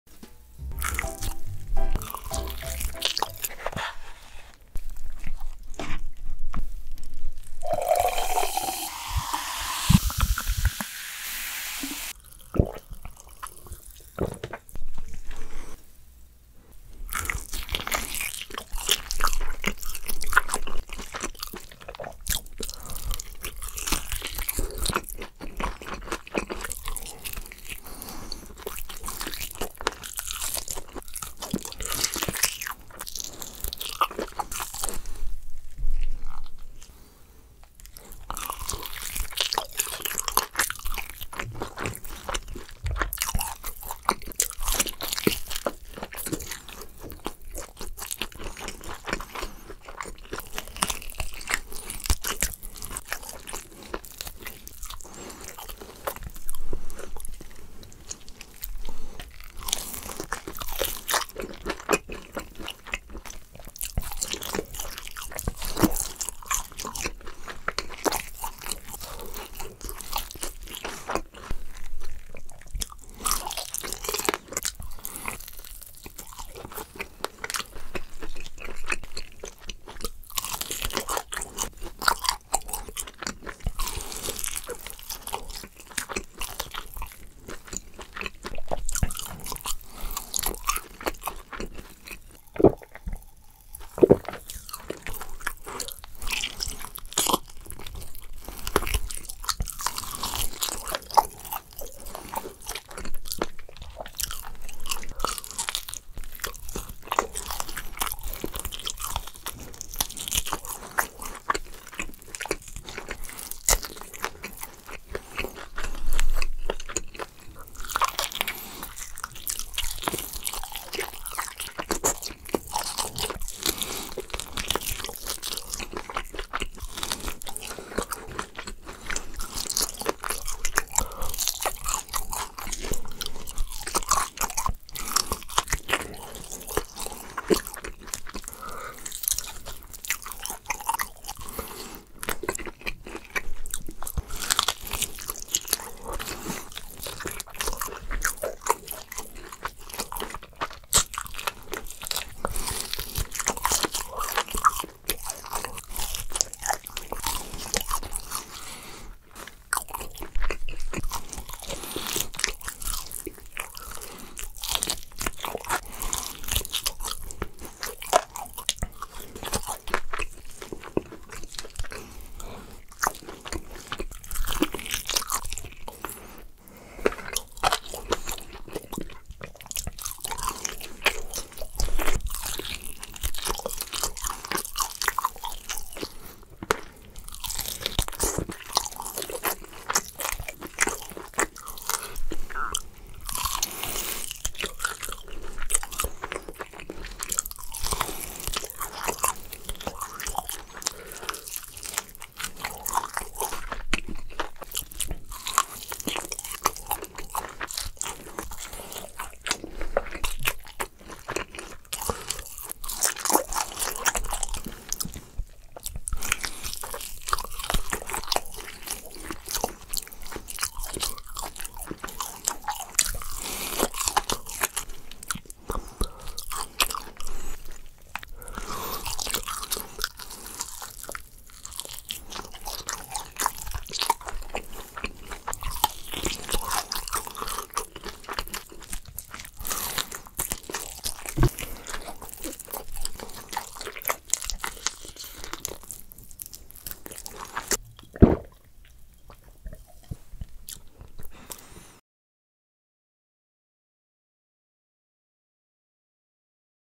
ASMR CHICKEN (KOREAN FOOD) EATING SOUNDS Mukbang チキン ayam मुर्गी pollo frango 鸡 ไก่ thịt gà
MUKBANG ASMR CHICKEN (KOREAN FOOD) EATING SOUNDS [NO TALKING] yum EATING SHOW

Today is CHICKEN (KOREAN FOOD)

★Mukbang Youtube
  Recommended to wear earphones for more vivid sound
 (There are subtitles)
 We hope you enjoy watching the video😊